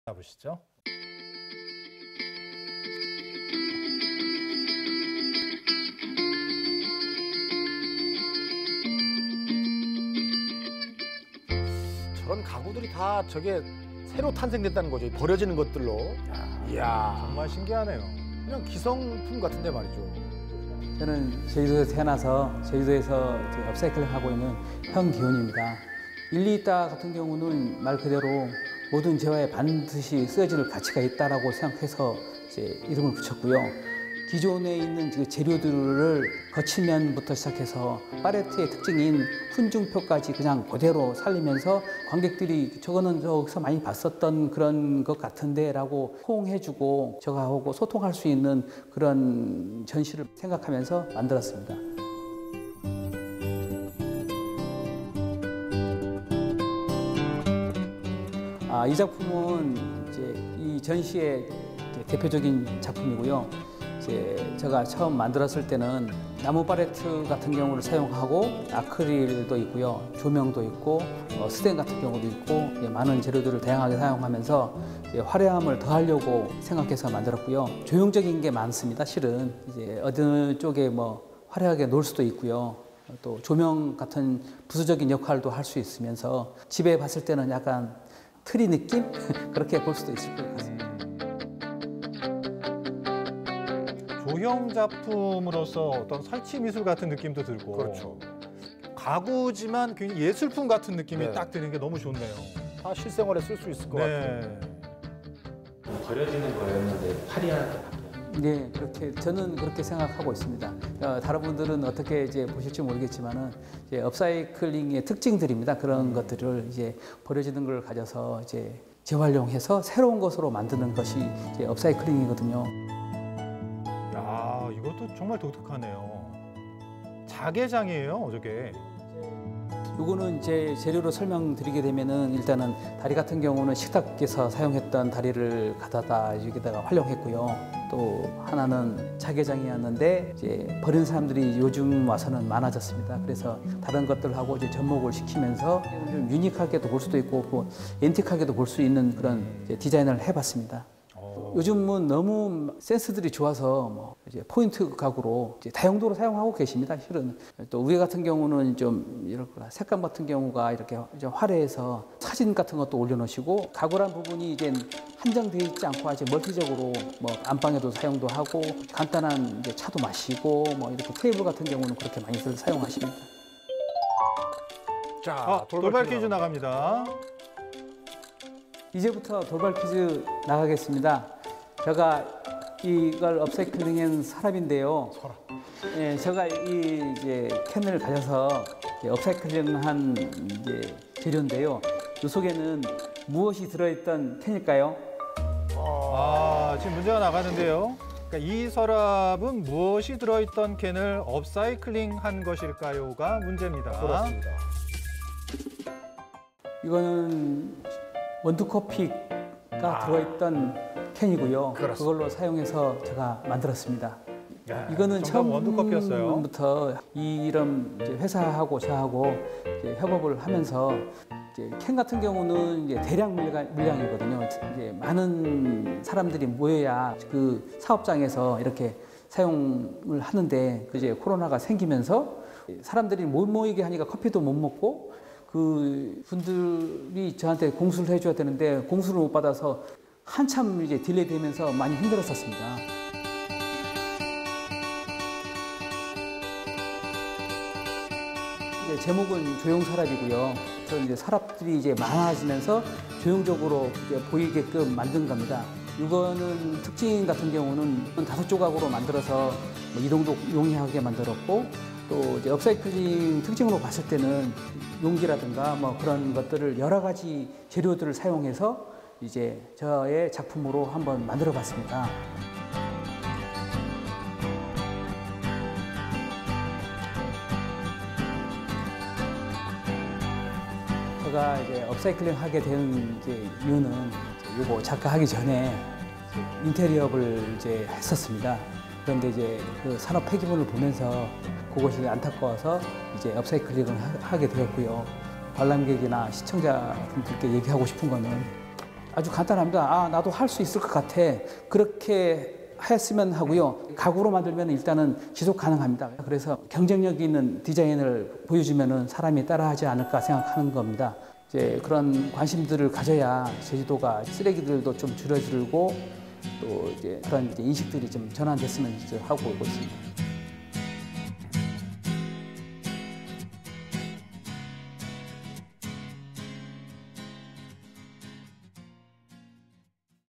문화_ 현기훈 업사이클링 가구전 '一理;있다' (와랑와랑TV)
현기훈 업사이클링 가구전 '一理;있다'
장소 : 심헌 갤러리
날짜 : ~ 2021년 5월 13일까지